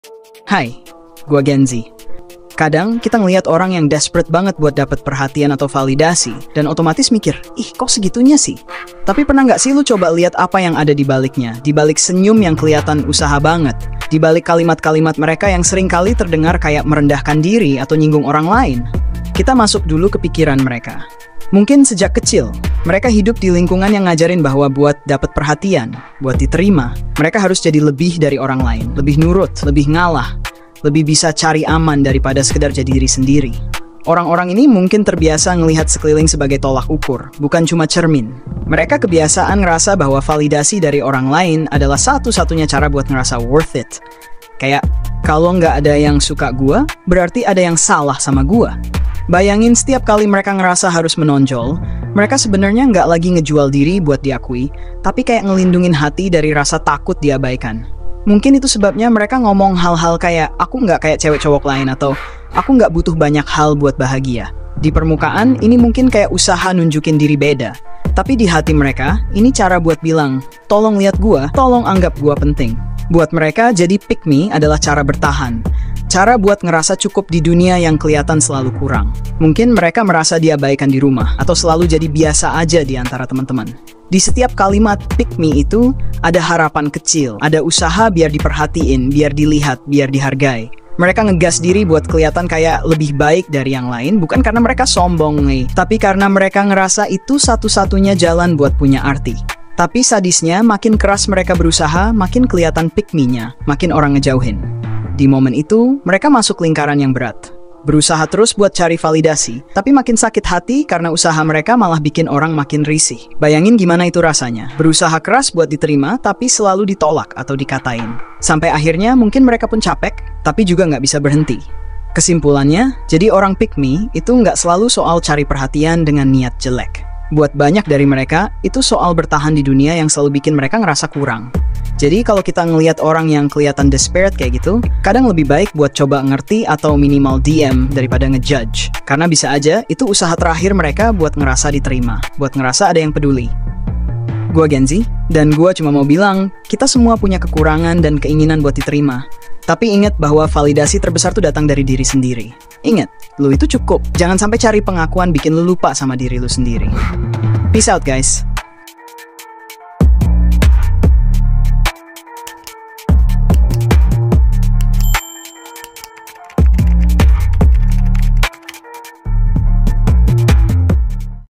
"Pickme" - Cerita Dibalik Attitude
Hai, gua Genzi. Gua ajak lu buat bayangin perspektif orang “pick me” dari sudut pandang mereka langsung. Kadang, kita ngeliat orang yang desperate banget buat dapet perhatian atau validasi, dan otomatis mikir, “Ih, kok segitunya sih?” Tapi, pernah nggak sih lu coba liat apa yang ada di baliknya, di balik senyum yang keliatan “usaha banget,” di balik kalimat-kalimat mereka yang seringkali terdengar kayak merendahkan diri atau nyinggung orang lain?

Kita masuk dulu ke pikiran mereka. Mungkin sejak kecil, mereka hidup di lingkungan yang ngajarin bahwa buat dapet perhatian, buat diterima, mereka harus jadi “lebih” dari orang lain—lebih nurut, lebih ngalah, lebih bisa “cari aman” daripada sekedar jadi diri sendiri. Orang-orang ini mungkin terbiasa ngelihat sekeliling sebagai tolak ukur, bukan cuma cermin. Mereka kebiasaan ngerasa bahwa validasi dari orang lain adalah satu-satunya cara buat ngerasa worth it, kayak, “Kalo nggak ada yang suka gua, berarti ada yang salah sama gua.”

Bayangin setiap kali mereka ngerasa harus menonjol, mereka sebenernya nggak lagi ngejual diri buat diakui, tapi kayak ngelindungin hati dari rasa takut diabaikan. Mungkin itu sebabnya mereka ngomong hal-hal kayak, “Aku nggak kayak cewek-cowok lain” atau “Aku nggak butuh banyak hal buat bahagia.” Di permukaan, ini mungkin kayak usaha nunjukin diri beda, tapi di hati mereka, ini cara buat bilang “Tolong liat gua, tolong anggap gua penting.”

Buat mereka, jadi “pick me” adalah cara bertahan, cara buat ngerasa cukup di dunia yang keliatan selalu kurang. Mungkin mereka merasa diabaikan di rumah, atau selalu jadi “biasa aja” di antara teman-teman. Di setiap kalimat “pick me” itu, ada harapan kecil, ada usaha biar diperhatiin, biar dilihat, biar dihargai. Mereka nge-gas diri buat keliatan kayak “lebih baik” dari yang lain bukan karena mereka sombong, tapi karena mereka ngerasa itu satu-satunya jalan buat punya arti.

Tapi, sadisnya, makin keras mereka berusaha, makin keliatan “pick me”-nya, makin orang ngejauhin. Di momen itu, mereka masuk lingkaran yang berat: berusaha terus buat cari validasi, tapi makin sakit hati karena usaha mereka malah bikin orang makin risih. Bayangin gimana itu rasanya—berusaha keras buat diterima, tapi selalu ditolak atau dikatain. Sampai akhirnya, mungkin mereka pun capek, tapi juga nggak bisa berhenti.

Kesimpulannya, jadi orang “pick me” itu nggak selalu soal cari perhatian dengan niat jelek. Buat banyak dari mereka, itu soal bertahan di dunia yang selalu bikin mereka ngerasa kurang. Jadi, kalo kita ngeliat orang yang keliatan desperate kayak gitu, kadang lebih baik buat coba ngerti atau minimal diem daripada nge-judge. Karena bisa aja, itu usaha terakhir mereka buat ngerasa diterima, buat ngerasa ada yang peduli.

Gua Genzi, dan gua cuma mau bilang, kita semua punya kekurangan dan keinginan buat diterima, tapi inget bahwa validasi terbesar tuh datang dari diri sendiri. Inget, lu itu cukup. Jangan sampe cari pengakuan bikin lu lupa sama diri lu sendiri. Peace out, guys!